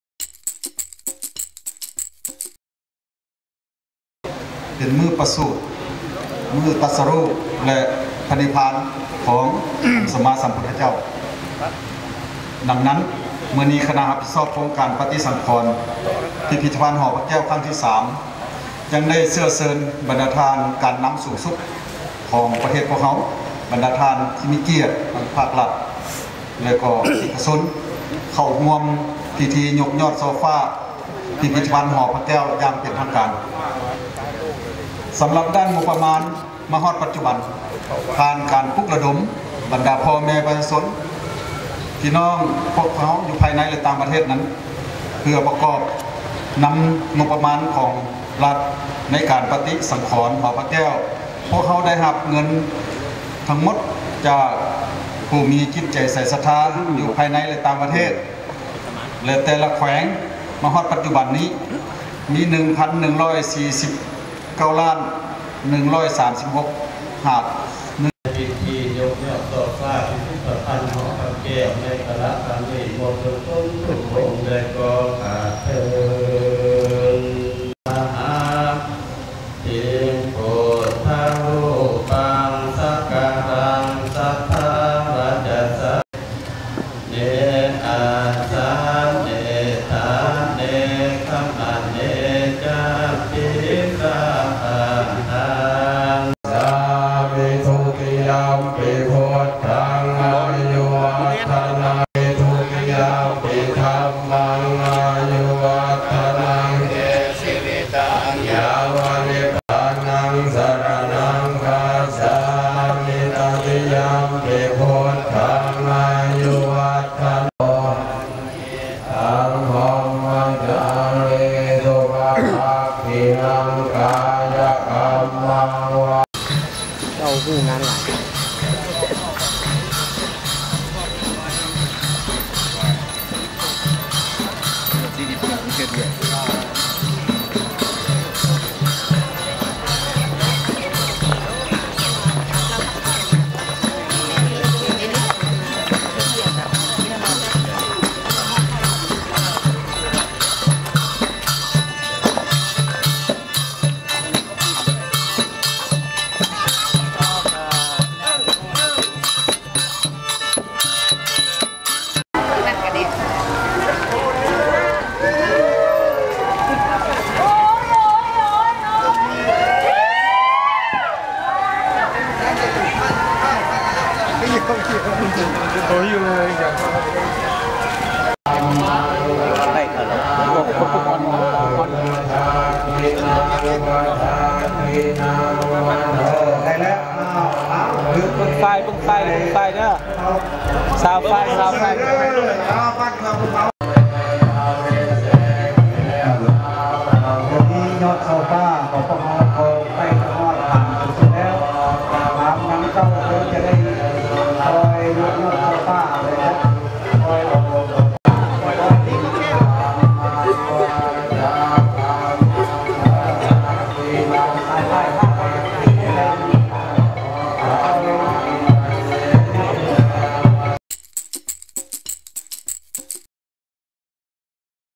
Renovated museum receives crowning glory
President Choummaly Sayasone and his wife on Saturday led a ceremony that saw 17 golden gable finials placed atop the renovated Hor Pha Keo Museum in Vientiane.

The restoration of the museum, one of the capital's most visited landmarks, is fully on track since beginning in December, although further public donations are needed to continue the work.
Speaking at the ceremony, which was attended by senior government officials, Vice Mayor of Vientiane Mr Saithong Keoduangdy said the restoration of the Hor Pha Keo Museum was important for Lao people because it sustained their fine traditions and culture.

“All Lao people are encouraged to contribute by making donations for the improvement of one of Vientiane's most important cultural sites,” he said.

The restoration of Hor Pha Keo is the third improvement project since the structure was first built in 1565 during the reign of King Setthathirath.

“The project is slated for completion by the end of this year,” Mr Saithong said.

The ceremony to place the ornamental gable finials in position began with a religious ceremony led by the President of the Lao Buddhist Fellowship, the Most Venerable Phong Samaleuk, and other senior monks from Vientiane temples.

This was followed by a parade of the gable finials around the museum three times, led by Deputy Prime Minister and Minister of Education and Sports Dr Phankham Viphavanh, Minister of Information, Culture and Tourism Prof. Dr Bosengkham Vongdara, Mayor of Vientiane Dr Sinlavong Khoutphaythoune, deputy ministers, the spouses of senior government officials, and invited Vientiane residents.

The gable finials were hoisted onto the rooftop by an electric winch.

So far, the restoration project has received more than 1.1 billion kip, US$15,793 and three kilogrammes of gold from public donations, according to the committee in charge of the project.

The committee estimates that more than 6.3 billion kip will be needed to purchase new materials to restore damaged sections of the museum.

More donations of materials and cash are being sought from members of the public both at home and overseas to ensure the museum is renovated to a high standard.

Hor Pha Keo is one of the oldest buildings in Vientiane and was used as a royal chapel during the time of the Lanexang Kingdom and also housed the precious Emerald Buddha (Pha Keo).

The building was destroyed when the Siamese (Thai) invaded Vientiane in 1779 and took the Emerald Buddha to Siam (Thailand), where it is housed in Wat Phra Kaew in Bangkok.

In 1826-29, the Siamese again invaded Vientiane and once more destroyed the temple after it had been rebuilt during the rule of King Anouvong in 1816.

Hor Pha Keo was again restored in 1936-42.

By Times Reporters
(Latest Update May 4, 2015)